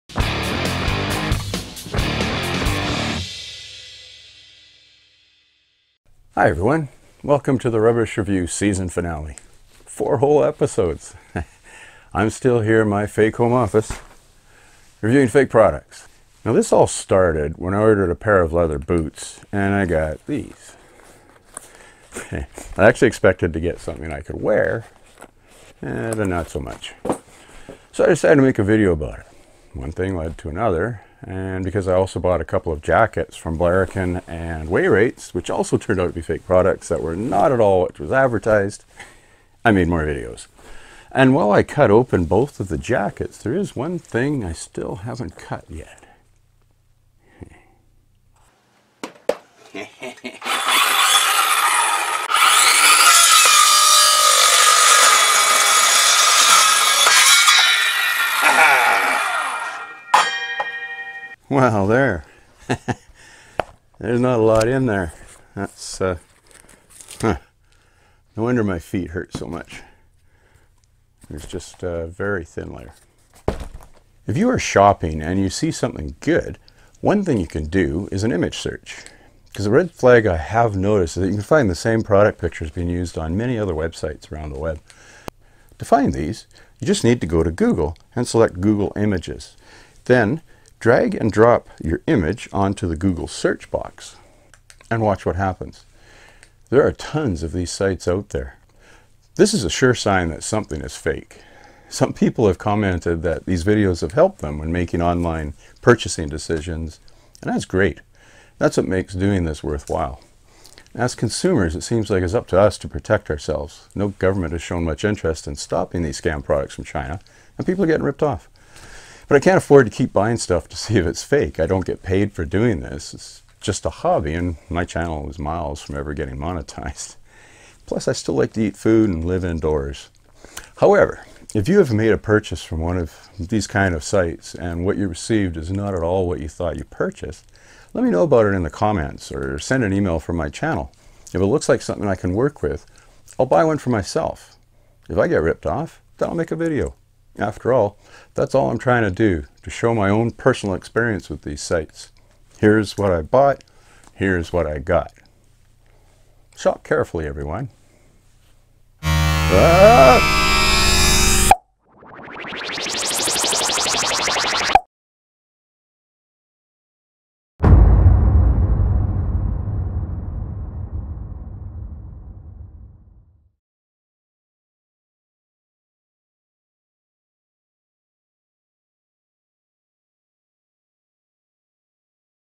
Wayrates and Blaroken – The Rubbish Review Season Finale S1E4
The big season finale! A quick look back at the products I purchased from Blaroken and Wayrates, and this time I use a power saw to investigate the quality.

I can't afford to keep buying things to see if they are fake, but if you have been sent fake products from one of these dodgy sites, let me know in the comments or send me an email from my channel. If it looks interesting, I will buy the same thing and see what I get...